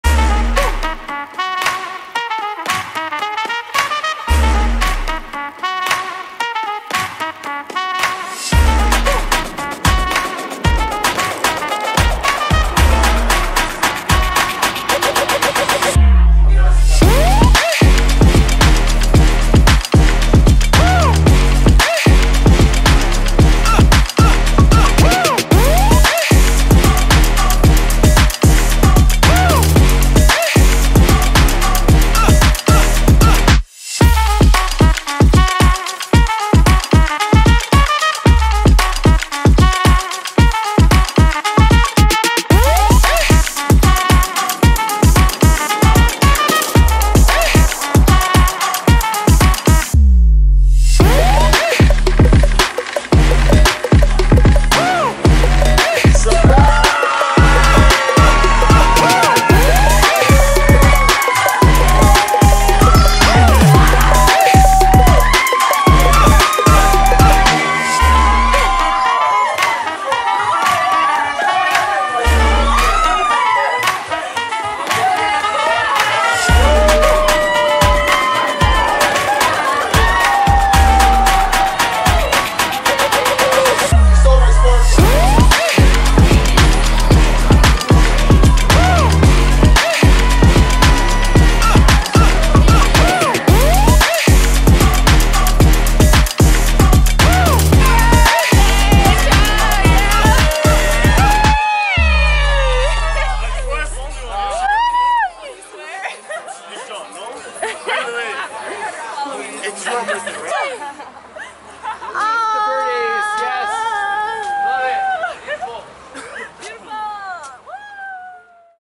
TOYBOX Birthday Party
Happy 21rst Birthday to a lovely woman, one of my best friends, and the greatest adventure buddy out there.